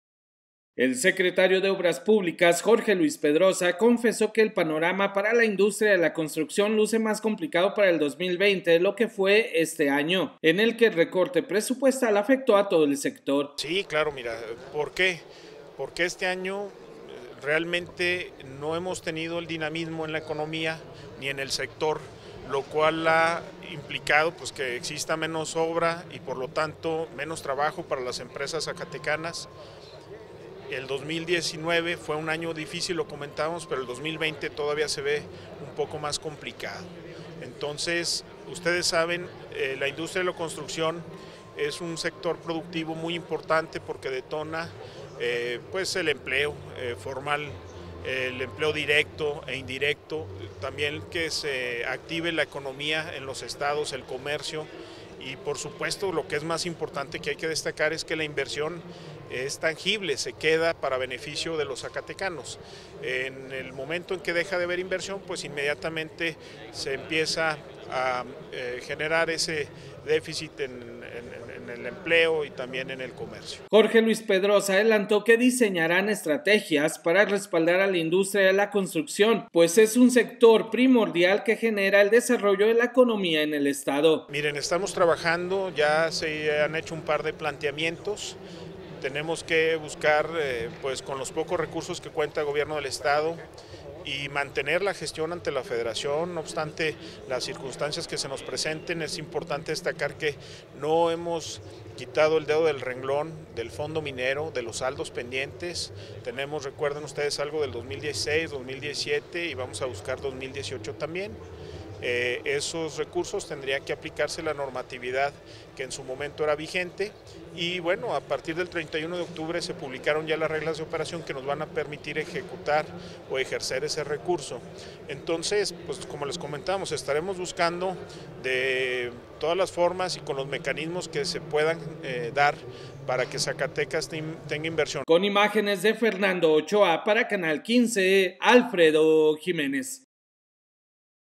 El 2020 Será Complicado para la Industria de la Construcción
La industria de la construcción detona empleo directo e indirecto, además genera economía, por lo que el anuncio del recorte a la obra pública complicará el panorama para el sector: Jorge Luis Pedroza.